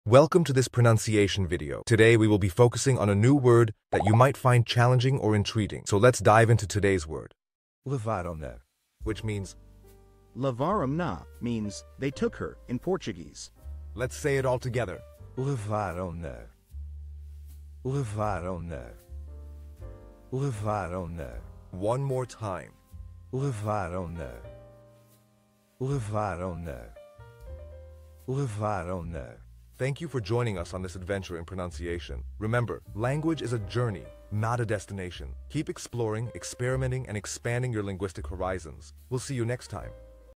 How to pronounce Levaram-na
Master the Pronunciation of 'Levaram-na Which Means took it' - which means : "They took it" Means "They Took Her" in Portuguese. 📝 with @PortuguesePronunciation 🎓🗣️ - [Your Guide to Portuguese]

Learn to pronounce 'Levaram-na' perfectly in Portuguese with our easy and engaging tutorial by @PortuguesePronunciation! 📖📽️ In this comprehensive video, we provide you with native speaker examples 🗣️👂 and phonetic breakdowns 🎵 to ensure you get the pronunciation just right.

How to say Led it in Portuguese